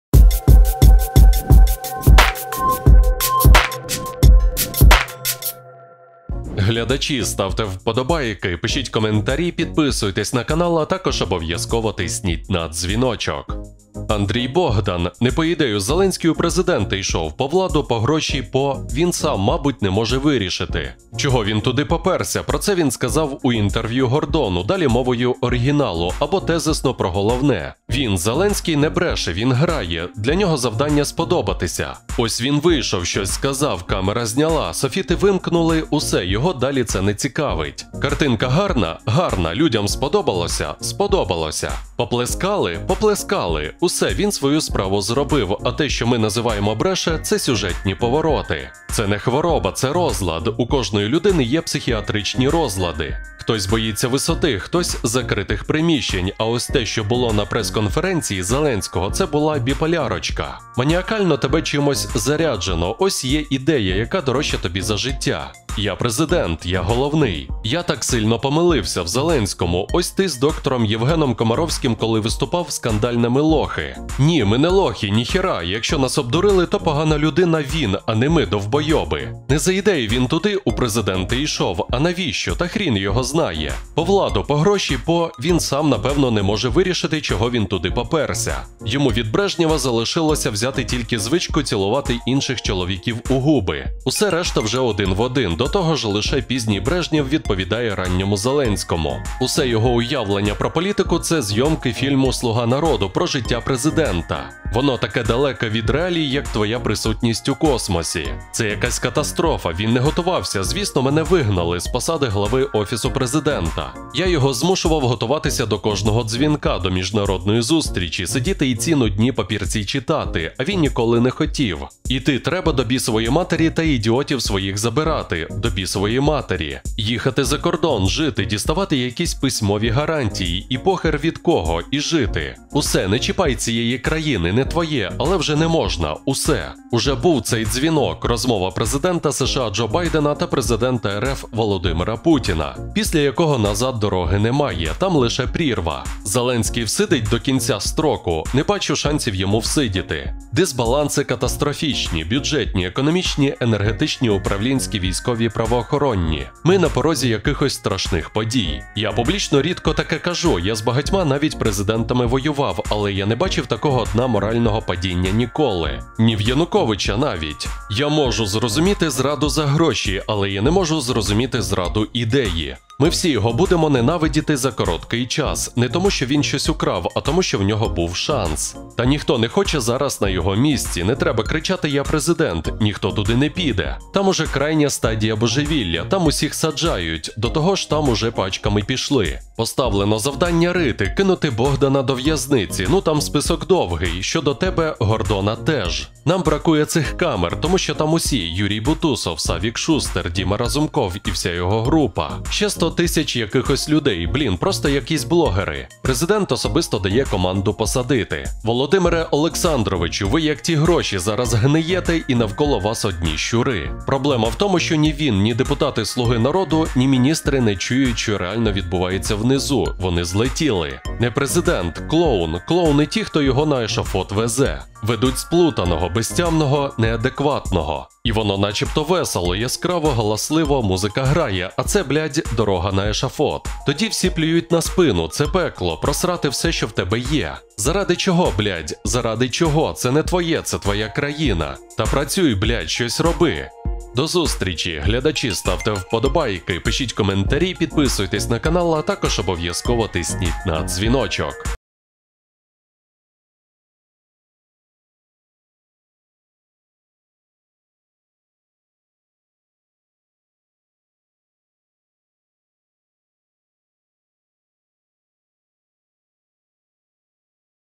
Бл#ть! Богдан попустив Зеленського: скоро буде втікати. Видав страшну таємницю
Андрій Богдан: Не по ідею Зеленський у президенти йшов. По владу, по гроші, по... Він сам, мабуть, не може вирішити, чого він туди поперся, про це він сказав у Інтервю Гордону, далі мовою оригіналу, або тезисно про головне.